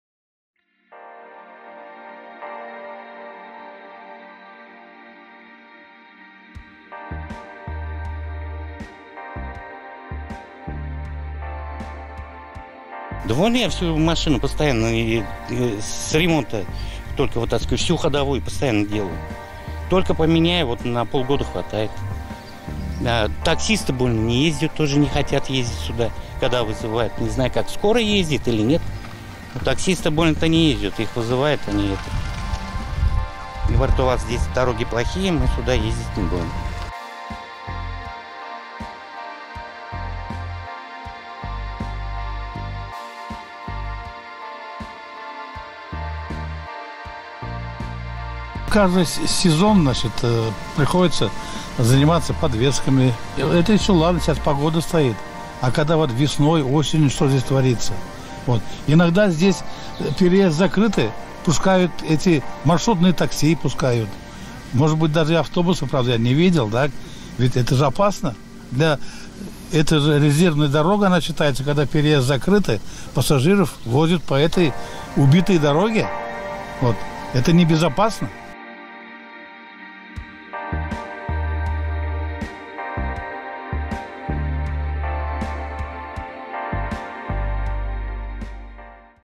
Жители Станции Калейкино жалуются на дорогу в село
Нужно объехать по обочине, - такие мысли первыми посетили нашу съемочную группу. Маршрут из точки А в точку Б превращается в квест для жителей Станции Калейкино. И даже обходные и объездные пути постепенно приходят в негодность. Разбитая дорога на Станции Калейкино последние два года на повестке дня сельчан.  Дорога настолько разбитая, что водители вынуждены проезжать ее по обочине.